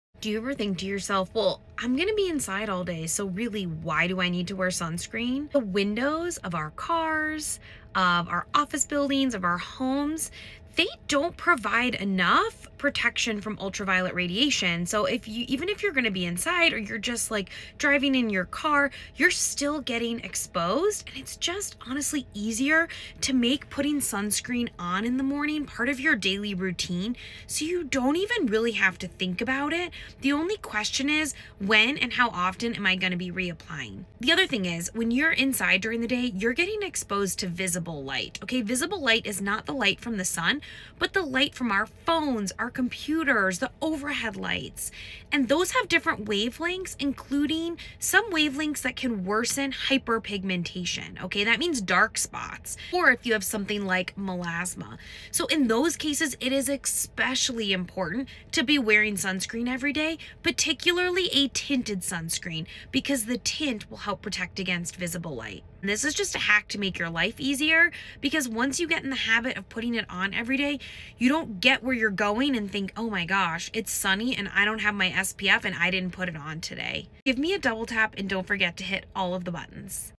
You’ve heard it before, but here’s the truth: yes, you need SPF indoors
You’ve heard it before, but here’s the truth: yes, you need SPF indoors. 💡 UVA rays (the aging rays) go right through windows — in your home, your office, your car. They don’t cause sunburn, so you won’t notice them… but they break down collagen, trigger pigmentation, worsen melasma, and speed up visible aging. Blue light from screens? The research is mixed, but for people with melasma or hyperpigmentation, sunscreen can help protect against dark spots. Bottom line: ✨ If you’re near a window ✨ Driving ✨ Or dealing with pigmentation issues …SPF is non-negotiable. 👉 Want simple, science-backed skin tips? Follow for daily derm advice!